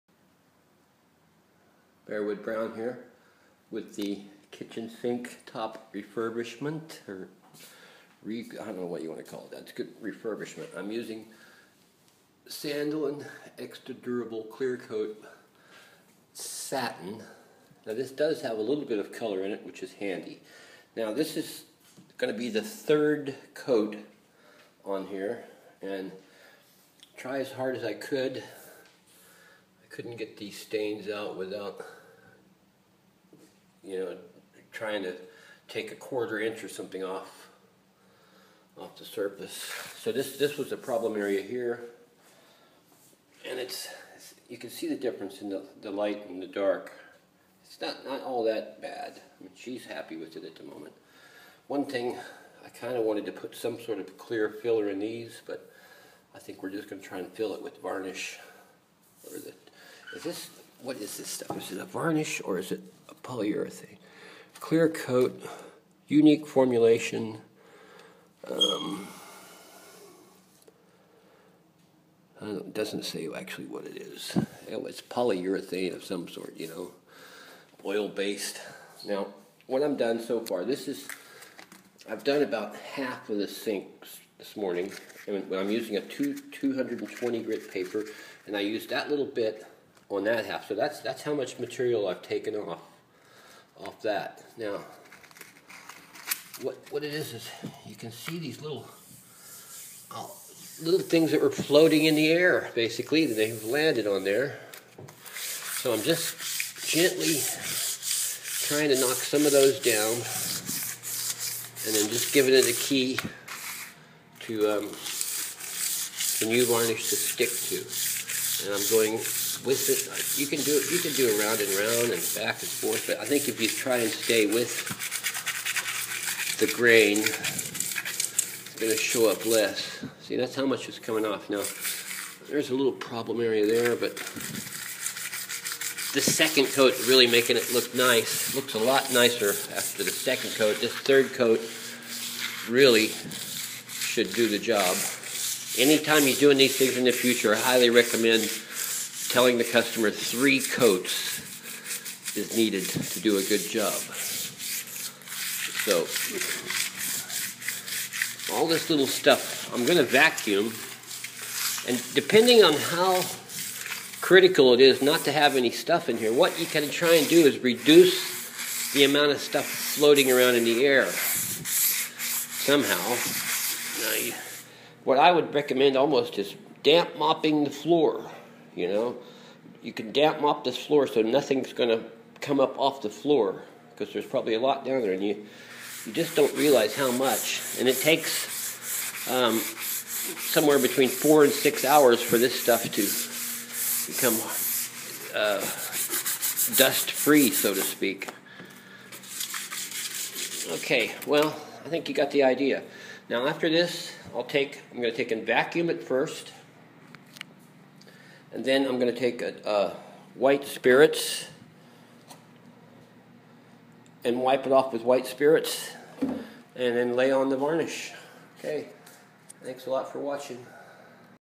Real Wood kitchen sink refinish + Bleaching part 3 finish coat
via YouTube Capture   Kitchen sink refurb, refinishing wood sink top, varnish counter top,sandolin,clear satin varnish, Varnish tips and tricks,
Dow corning sealant,